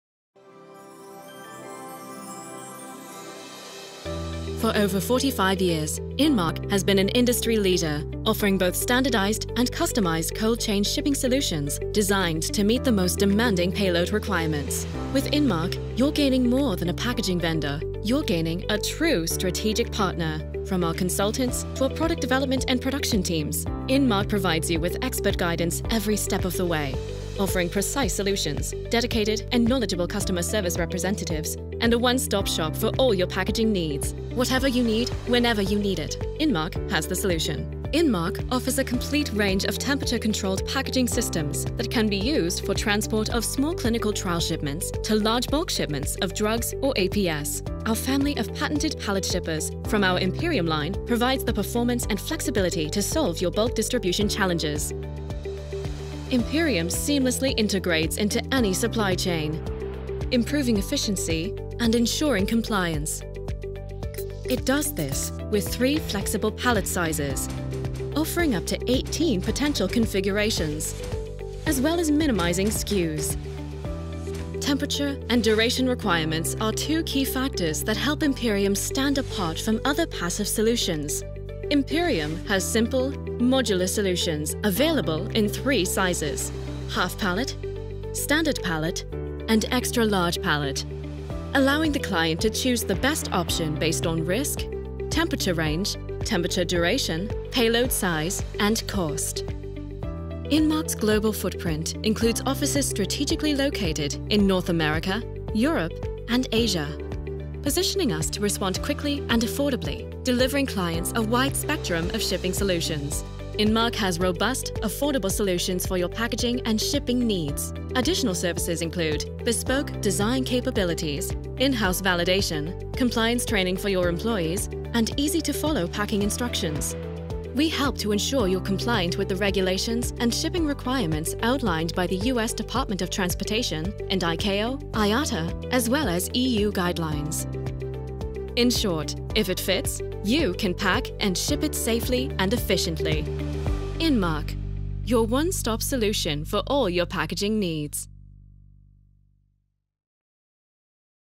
Inmark, Imperium Shipping Solutions
Inmark's Imperium™ family of patent-pending pallet shippers provides the performance and flexibility to solve your bulk distribution challenges.

Effectively manage and mitigate risk by tuning to your organization's specific risk profile(s), cost targets and business objectives.

By leveraging patent-pending design and advanced materials, as well as simple, modular, and intuitive design, Imperium family seamlessly integrates into any supply chain, improving efficiency and ensuring compliance.